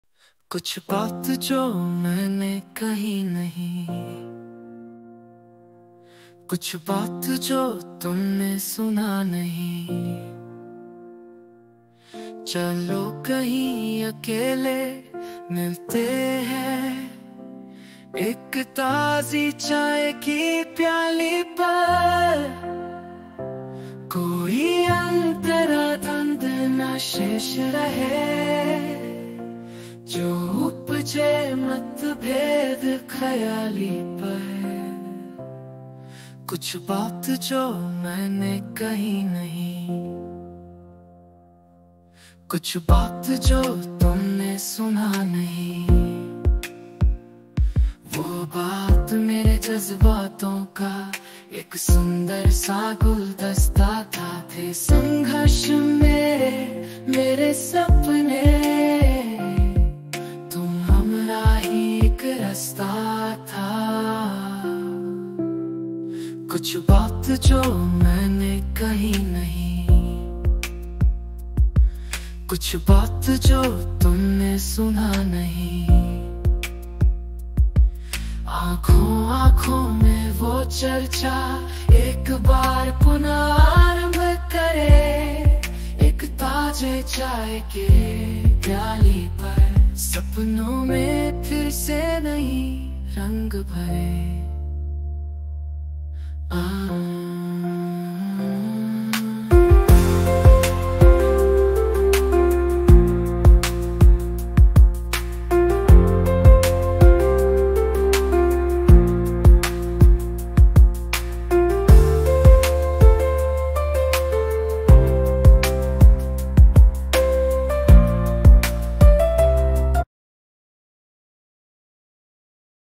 कुछ बात
कुछ बात जो मैंने कही नहीं, कुछ बात जो तुमने सुना नहींचलो कहीं अकेले मिलते हैं ,एक ताज़ी चाय की प्याली परकोई अंतर्द्वंद  न शेष रहे, जो उपजे मतभेद ख्याली पर

कुछ बात जो मैंने कही नहीं,कुछ बात जो तुमने सुना नहींवो बात मेरे जज़्बातों kaएक सुंदर सा गुलदस्ता थाथे संघर्ष मेरे, मेरे सपनेतुम हमराही, एक रस्ता था

कुछ बात जो मैंने कही नहीं,कुछ बात जो तुमने सुना नहींआंखों आंखों में वो चर्चा एक बार पुनः आरंभ करेंएक ताजे चाय के प्याली परसपनों में फिर से नई रंग भरें।